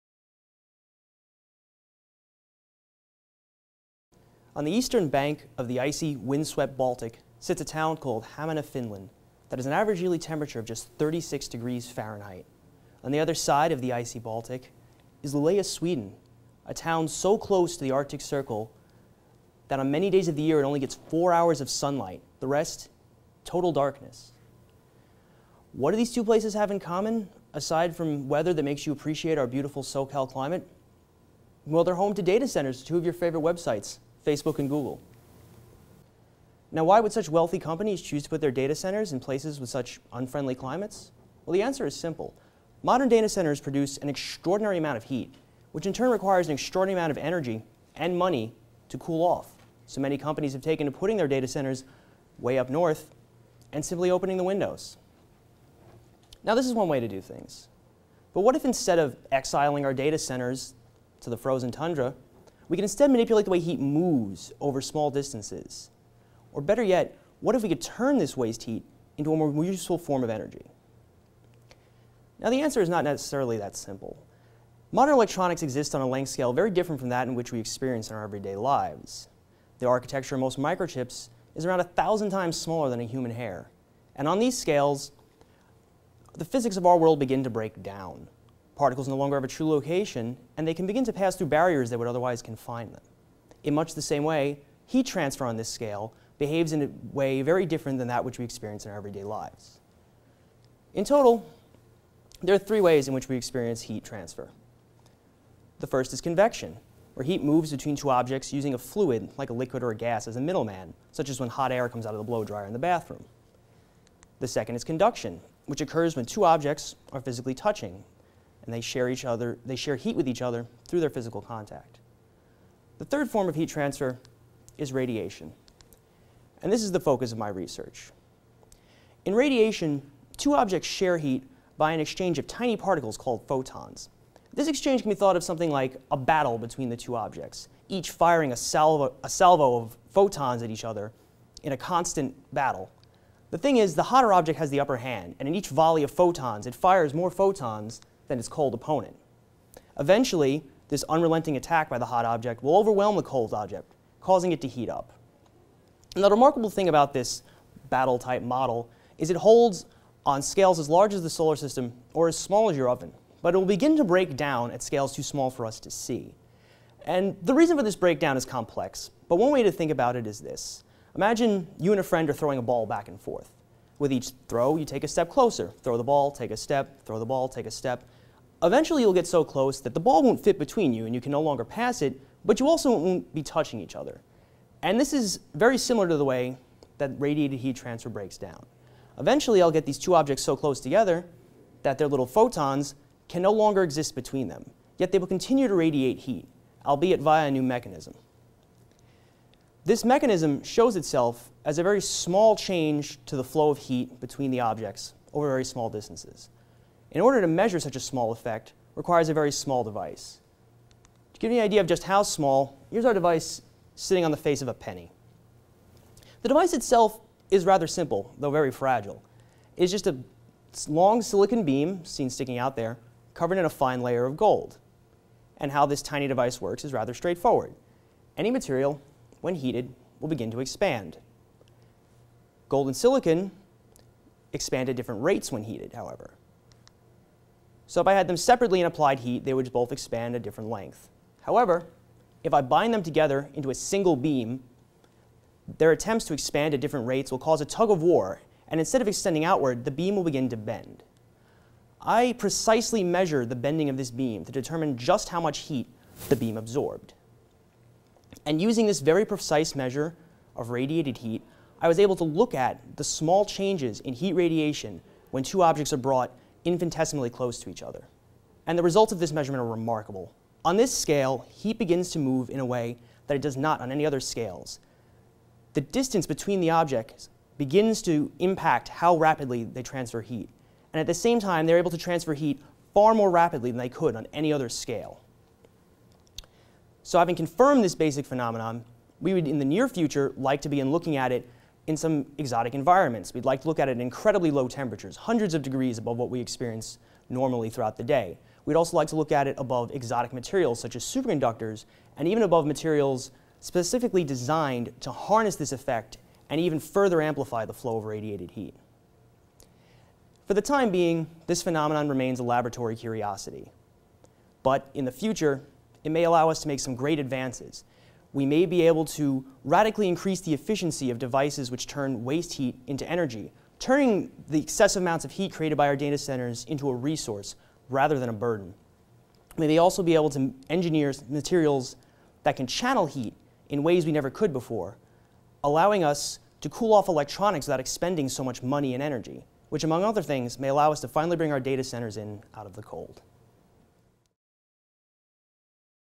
Presentation
Using skills learned in a class taught by Loh Down on Science's own Sandra Tsing Loh, I describe my PhD research and its impact in a talk meant to be both entertaining and informative. This talk describes research performed in the Low Temperature Physics Lab at UC Irvine.